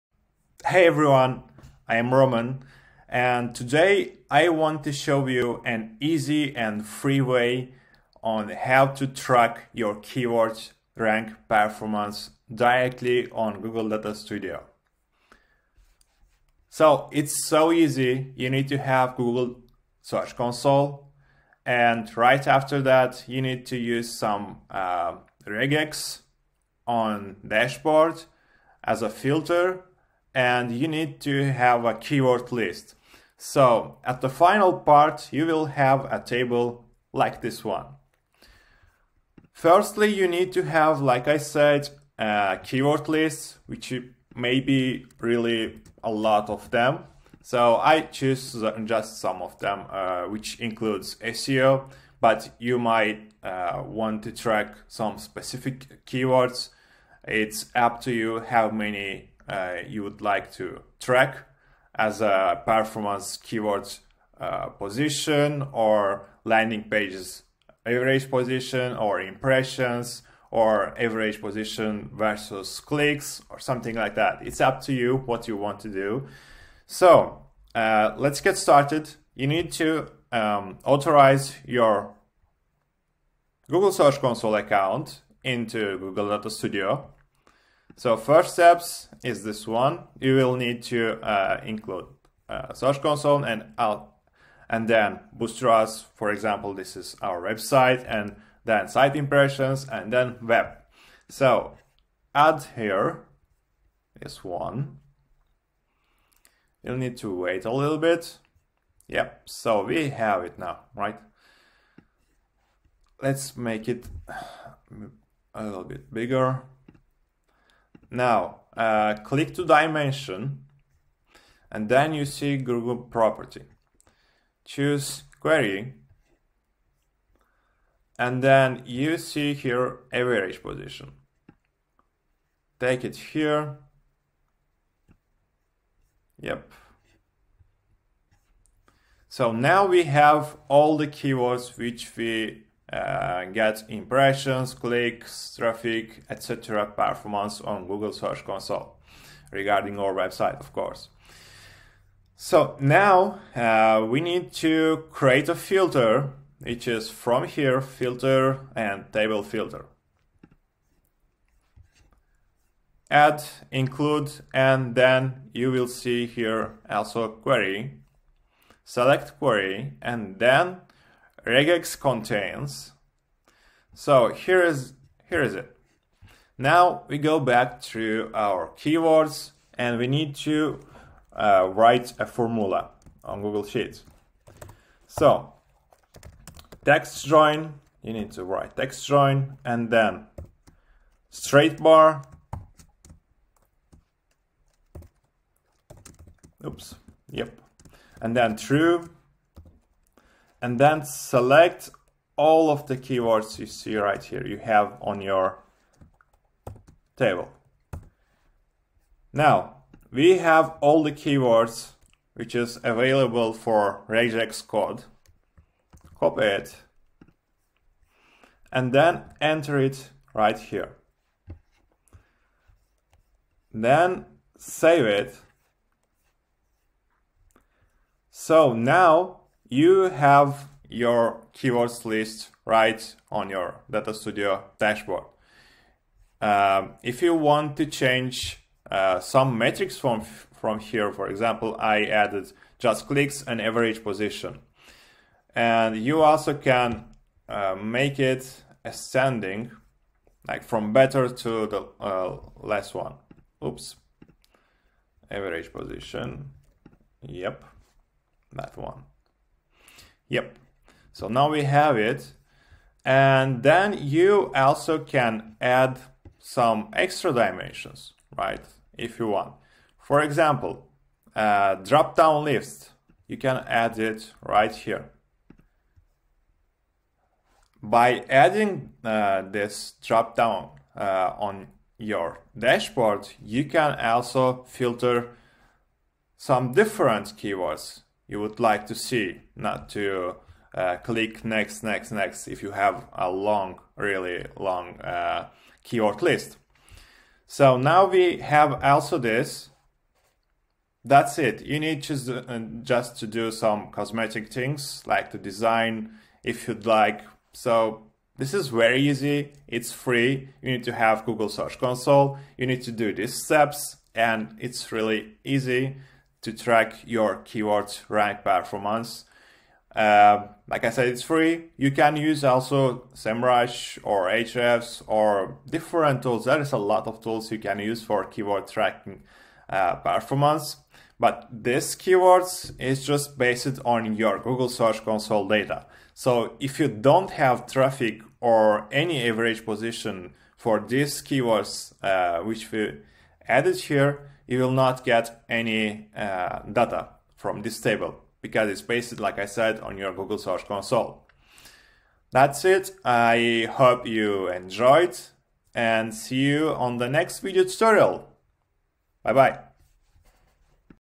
Track Your Keyword Ranking Performance Easily by Using Google Data Studio & Search Console
In this video tutorial, you will learn how to track organic ranking performance for any keyword and how many keywords you want, for free!

Using Google Data Studio, you can easily connect your Google Search Console website property to the report. This will open the gate to create any filter you would like to have for your insights.

Our Director of SEO, Roman Adamita, in this video, showed you how to create a filter by using Regex that will show you the ranking of the queries you would like to check.

Timestamps:
00:00 - Intro
00:16 - Example keyword ranking report created on Google Data Studio
00:40 - The example keyword list you should prepare for including in the report
01:19 - How to connect Google Search Console to Google Data Studio?
02:37 - How to create a filter on Google Data Studio by using regex?
03:00 - What kind of formula to use to collect all keywords that will be available for regex?